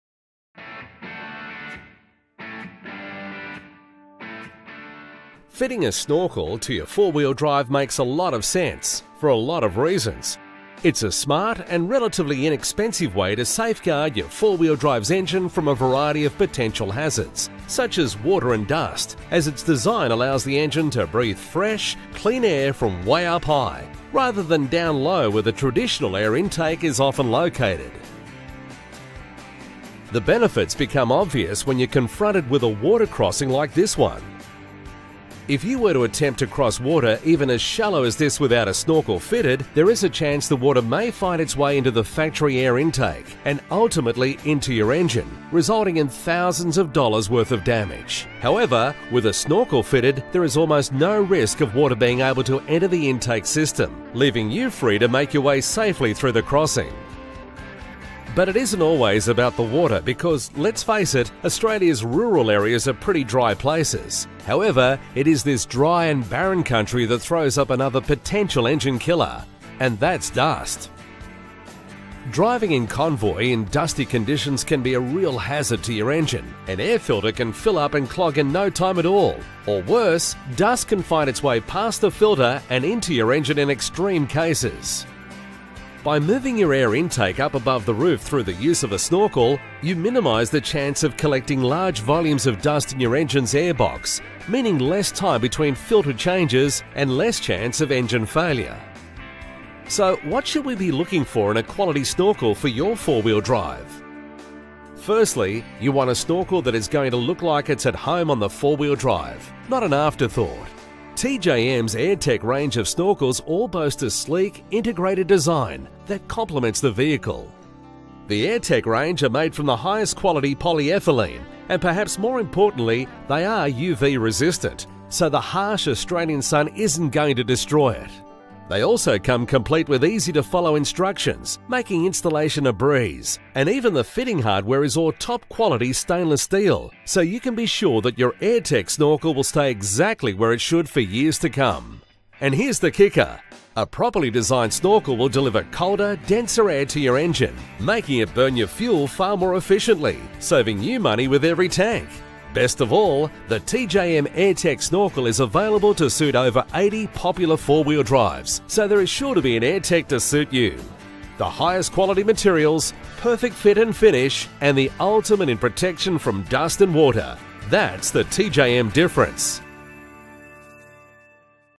TJM 4x4 Equipped - Airtec Snorkel
If you’re serious about four wheel driving you won’t get far without a snorkel. Give your engine cleaner, cooler air, free of dust and moisture with the TJM Airtec snorkel range. Built from the strongest, colour fast UV polymer material and designed to fit the natural shape of each vehicle. Your vehicle will not only run better but look good to.